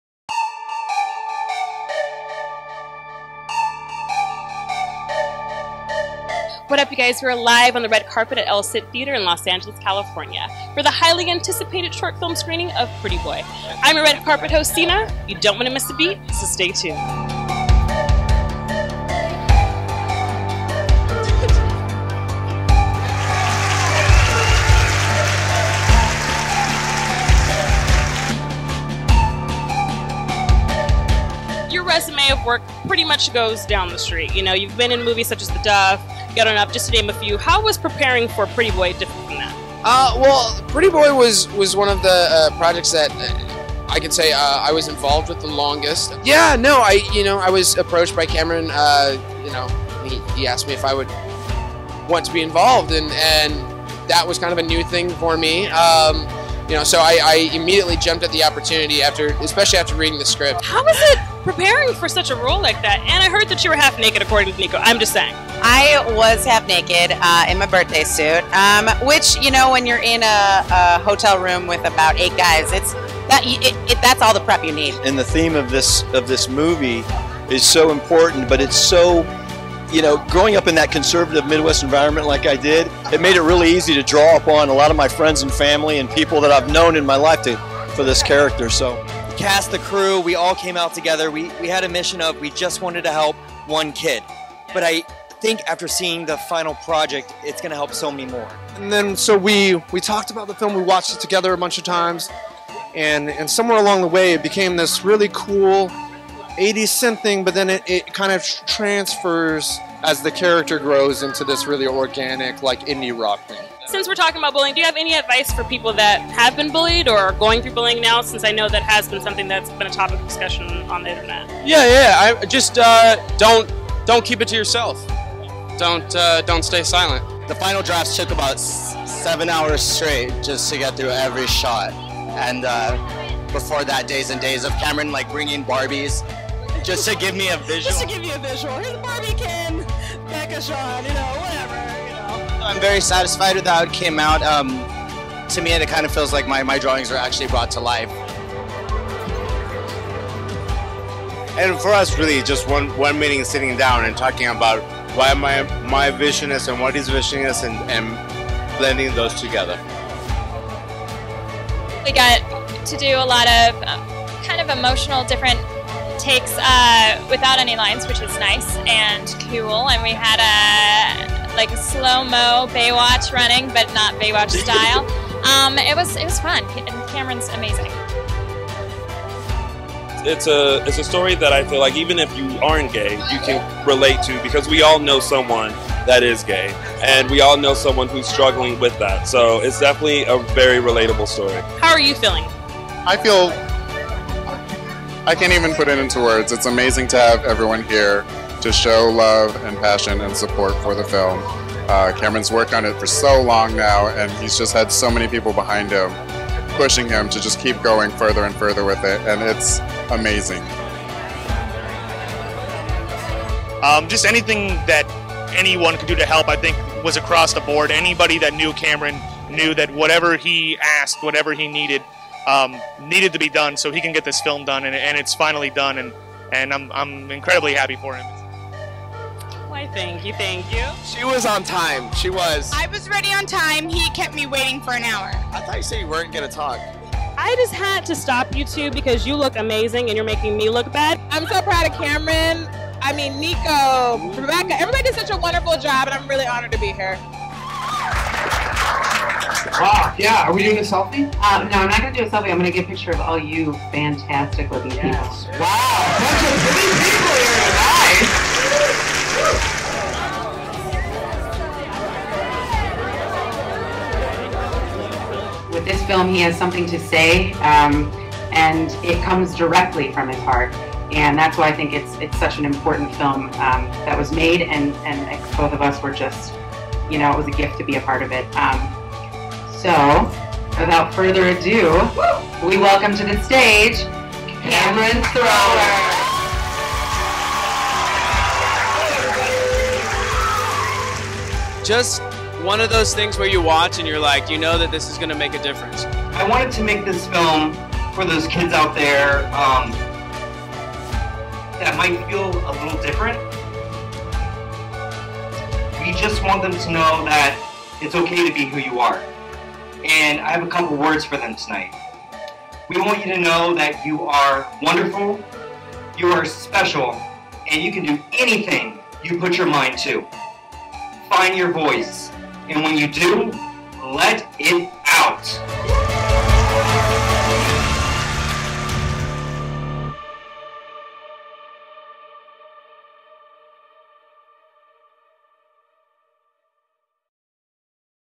"PRETTY BOY" Red Carpet Private Screening
On March 25th, 2015 Pretty Boy took over the Historic "El Cid" Theater in Los Angeles, CA for the much anticipated film, Pretty Boy. Go inside the private screening & onto the red carpet with our beautiful red carpet host, Seena Ali. She interviews "Pretty Boy" Cast, crew, and special guests. Be sure to look out for "Pretty Boy" at a festival near you!